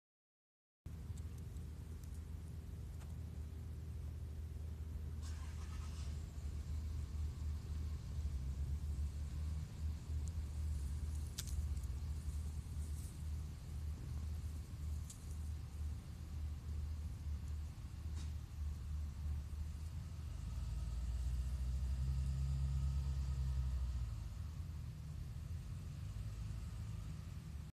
Helping a giant Darner Dragonfly drink water during the California drought
Helping a sick giant Darner Dragonfly that I found at night drink some water during the Southern California drought.